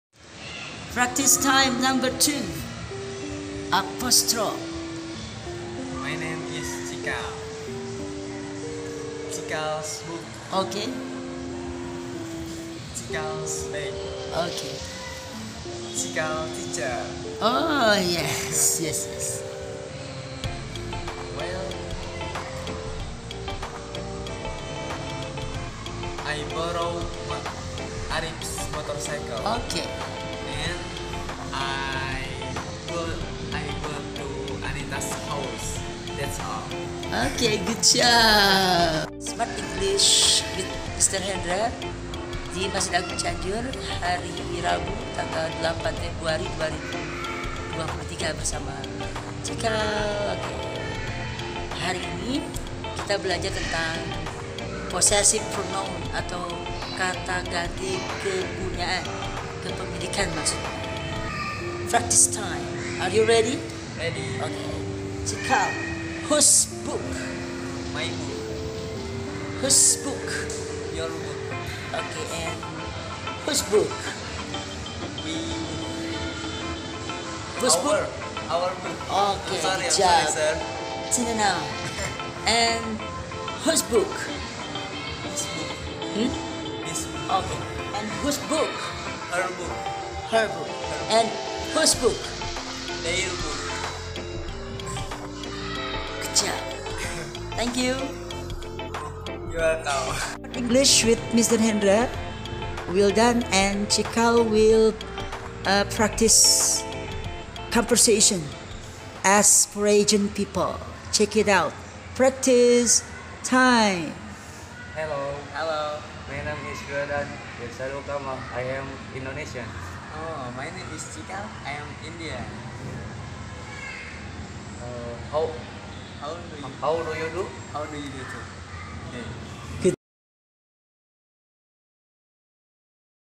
Les privat bahasa Inggris mengenai PRAKTEK percakapan POSSESIVE PRONOUN (Kata ganti kepunyaan benda)
Assalamualaikum sahabat setia channel YouTube SMART ENGLISH WITH MR HENDRA berikut video Pembelajaran bahasa Inggris untuk pemula mengenai bagaimana memulai percakapan dengan orang asing